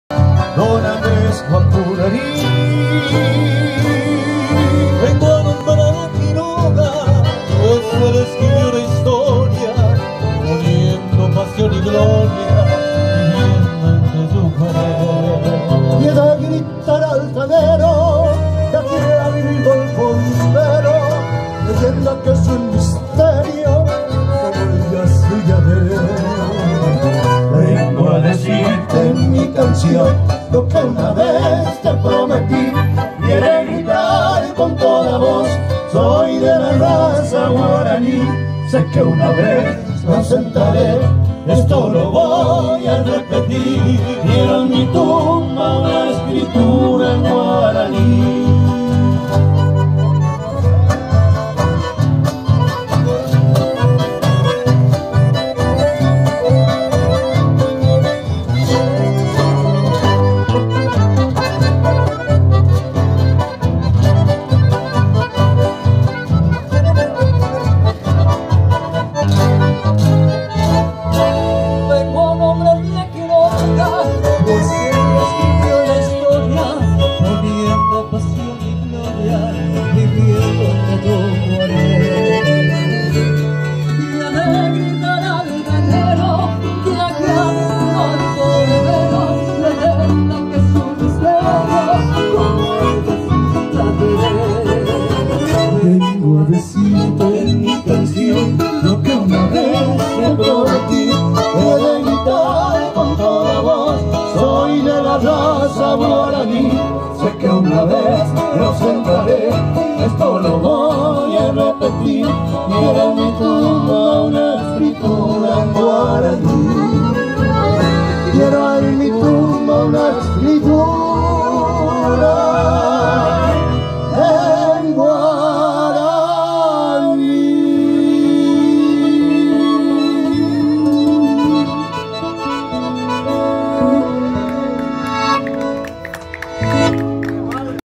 8/5/2021 POSADAS:DESDE LA PLAZA SANMARTINIANA VILLA CABELLO HOY ORQUESTA FOLCLÓRICA MUNICIPAL !
8 DE MAYO 2021 DESDE LA PLAZA SANMARTINIANA VILLA CABELLO HOY ORQUESTA FOLCLÓRICA MUNICIPAL RUMBO A SUS 25° ANIVERSARIO!CIUDAD DE POSADAS ,LOCUCION EL SEÑOR ROBERTO RETORI , EL SONIDO A CARGO DE LUZ Y SONIDOS SIEMPRE JUSTO OSCAR GONZALEZ, GAUCHO VIRUTA